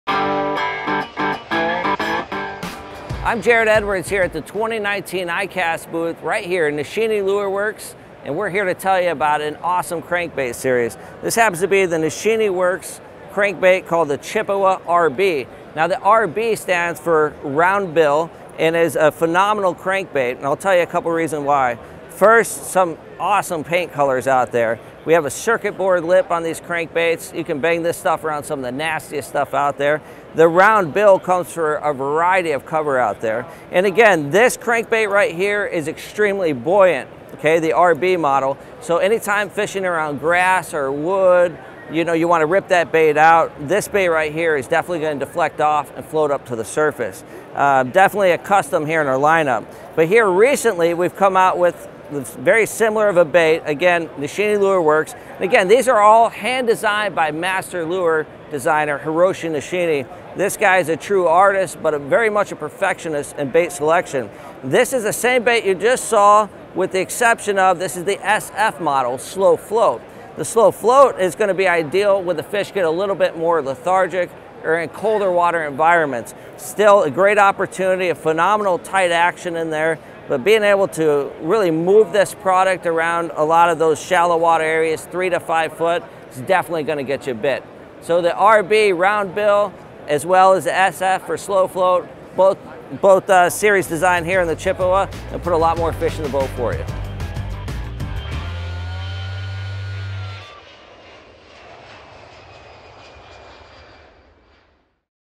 Nishine Lure Works Chippawa RB Crankbait & RB Slow Float Crankbait | iCast 2019
TW discusses the Nishine Lure Works Chippawa RB Crankbait & RB Slow Float Crankbait at the 2019 ICAST Show in Orlando, Florida.